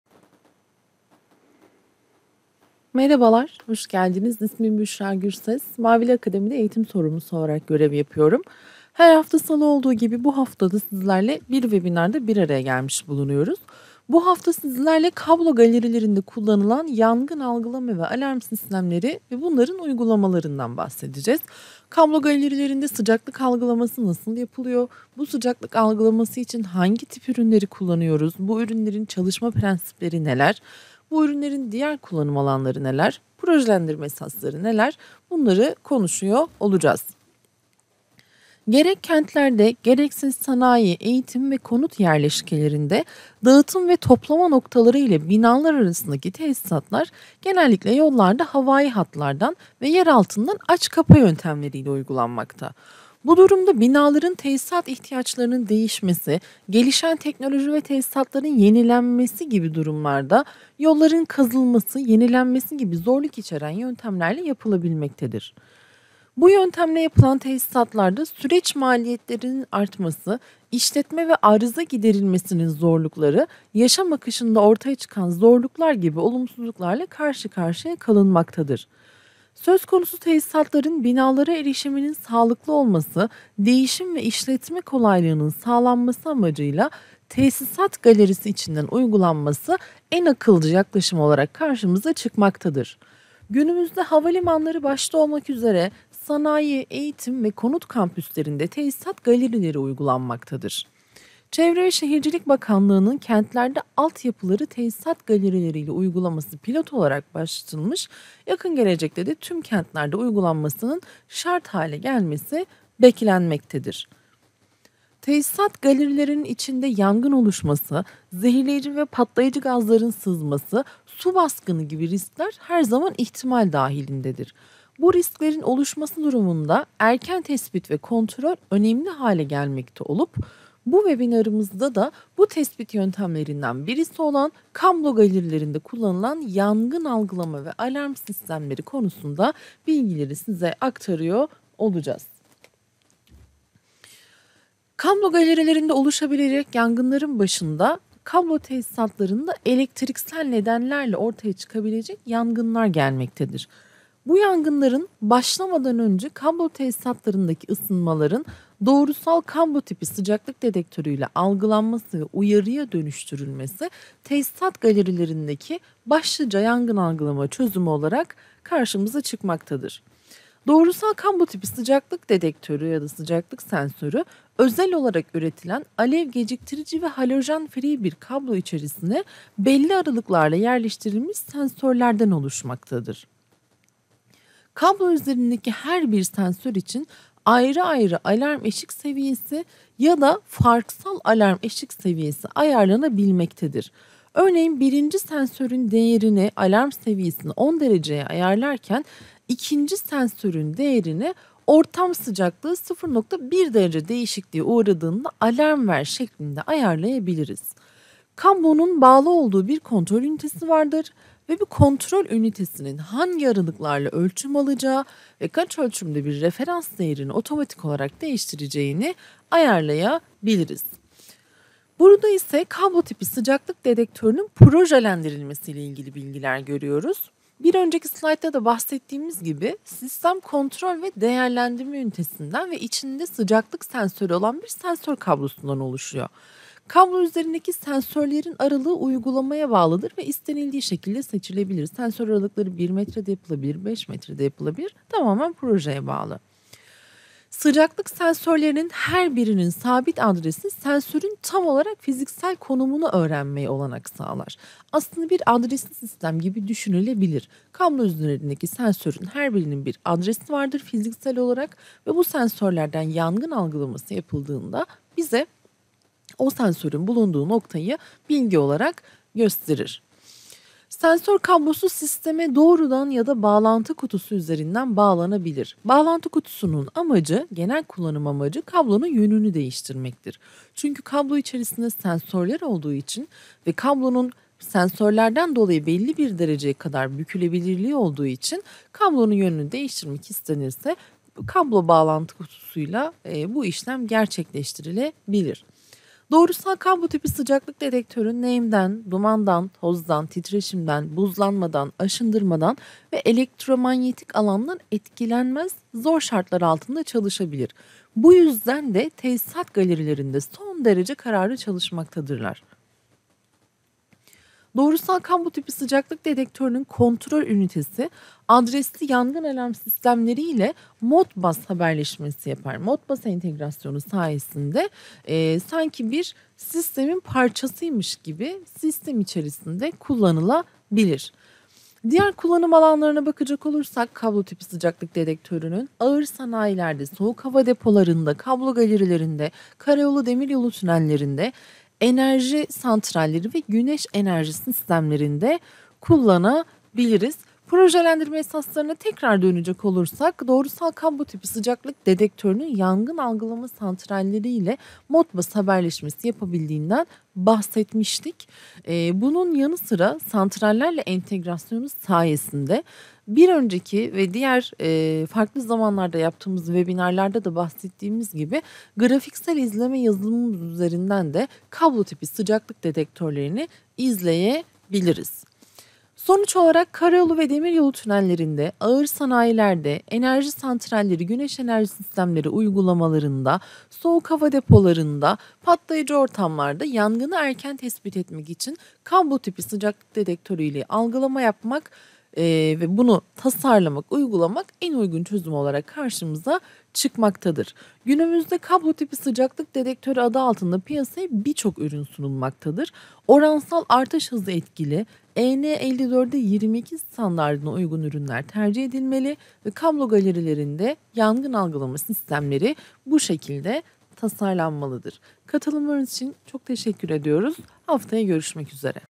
Akademi Webinar: Kablo Galerilerinde Kullanılması Gereken Yangın Algılama Sistemleri
Gerek kentlerde gerekse sanayi, eğitim ve konut yerleşkelerinde dağıtım ve toplama noktaları ile binalar arası tesisatların; binalara erişiminin sağlıklı olması, değişim ve işletme kolaylığının sağlanması amacıyla tesisat galerisi içerisinden uygulanması sonucu ortaya çıkan kablo galerilerinde yangın koruması amacıyla yapılması gerekenler anlatılmaktadır.